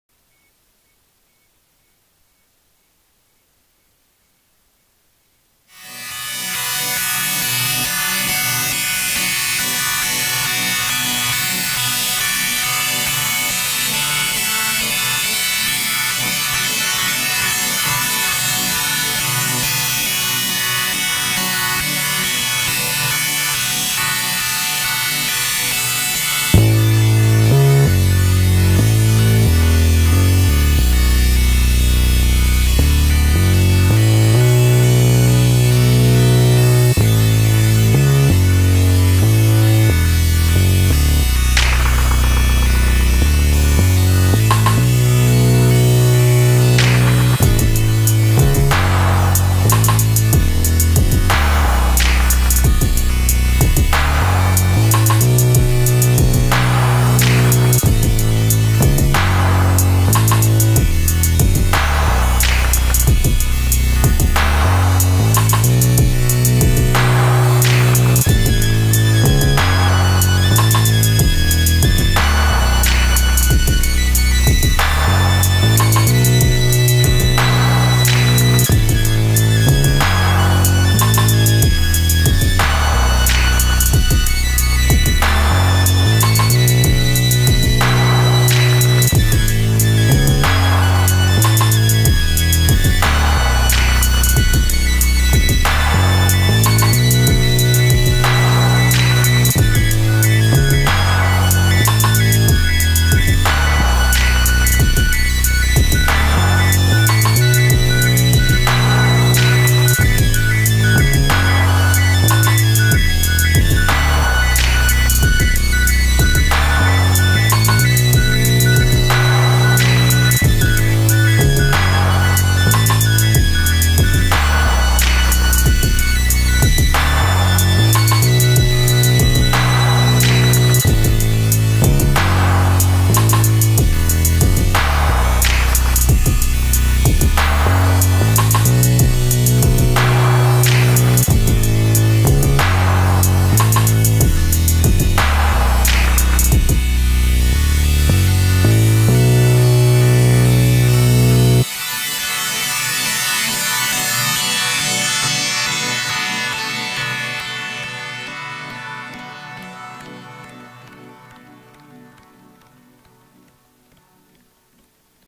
Beats From Last Night - 20180712
don't call it a comeback.  just out here on tour, making beats on the water.  balancing work and music has been especially tough this summer.  more soon.  maybe.